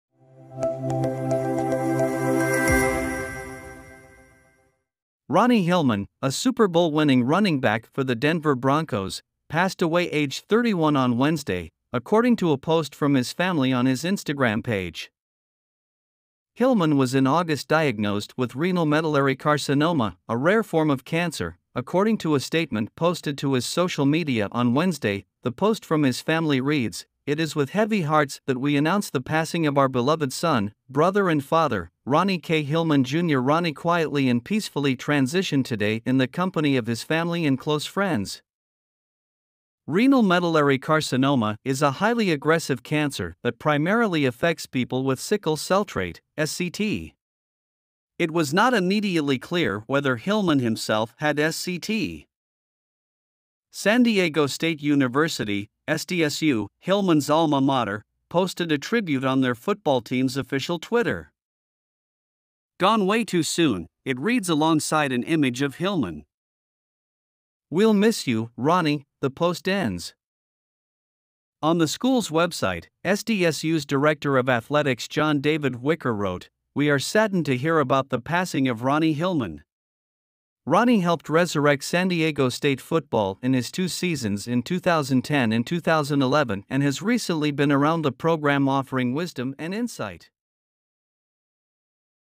Super Bowl winner Ronnie Hillman dies aged 31 after cancer battle
Ronnie Hillman, a Super Bowl-winning running back for the Denver Broncos, passed away aged 31 on Wednesday, according to a post from his family on his Instagram page.

Hillman was in August diagnosed with renal medullary carcinoma, a rare form of cancer, according to a statement posted to his social media on Wednesday.The post from his family reads, "It is with heavy hearts that we announce the passing of our beloved son, brother & father, Ronnie K Hillman Jr.Ronnie quietly and peacefully transitioned today in the company of his family and close friends."

Renal medullary carcinoma is a highly aggressive cancer that primarily affects people with sickle cell trait (SCT). It was not immediately clear whether Hillman himself had SCT.

San Diego State University (SDSU), Hillman's alma mater, posted a tribute on their football team's official Twitter.

"Gone way too soon," it reads alongside an image of Hillman.

"We'll miss you, Ronnie," the post ends.

On the school's website, SDSU's director of athletics John David Wicker wrote, "We are saddened to hear about the passing of Ronnie Hillman. Ronnie helped resurrect San Diego State football in his two seasons in 2010 and 2011 and has recently been around the program offering wisdom and insight.